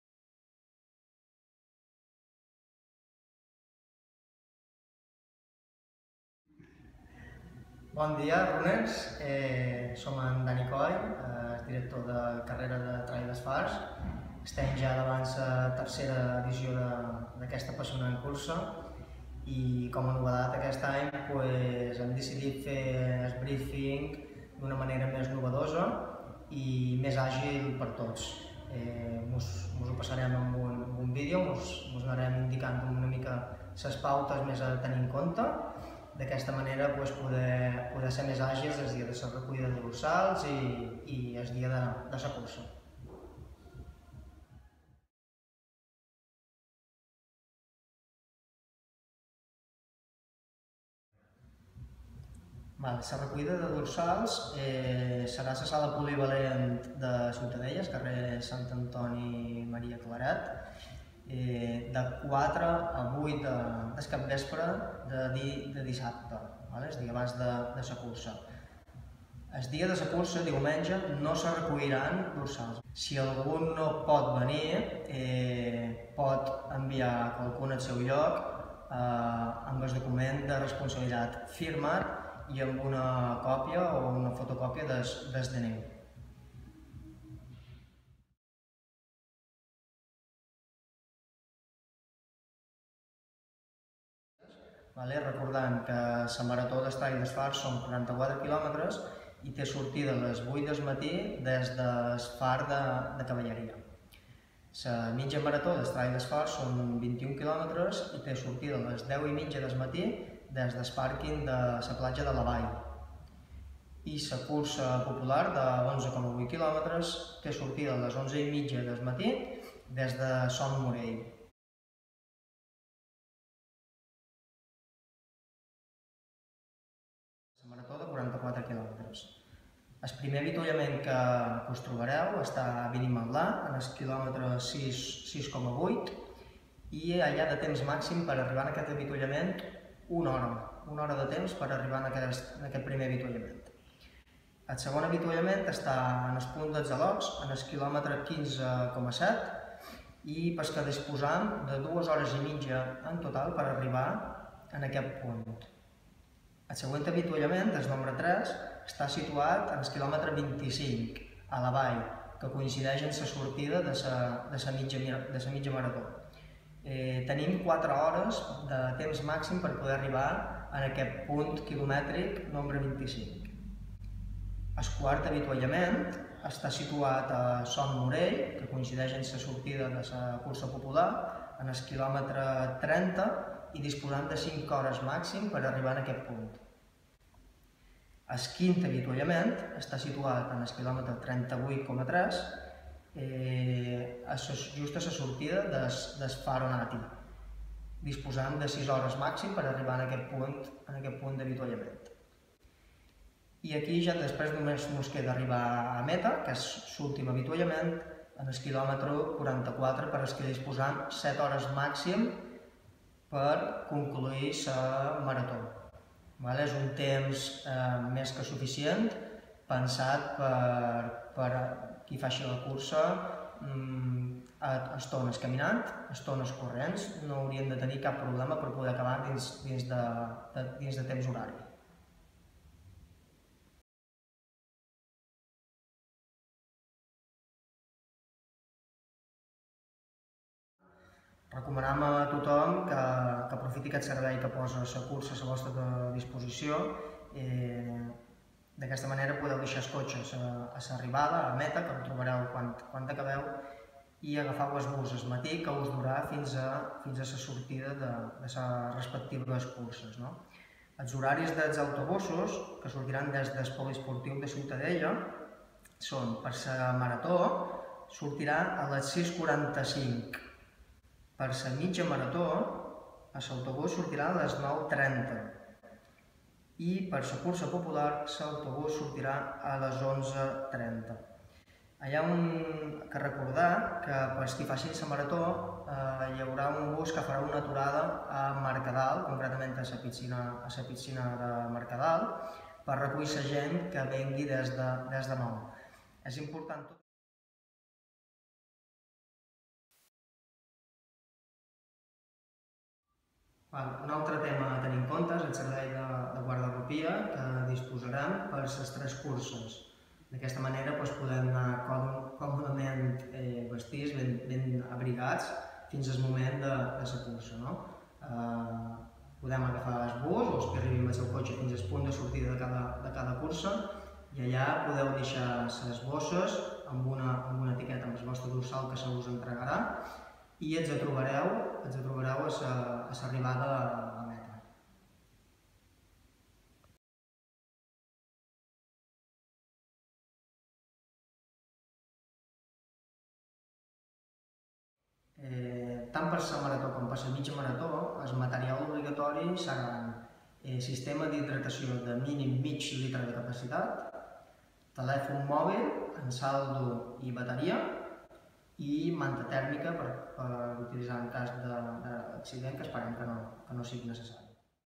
Brieffing Trail Dels Fars
El director de carrera, Dani Coll, explica algunos aspectos a tener en cuenta sobre la Compressport Trail dels Fars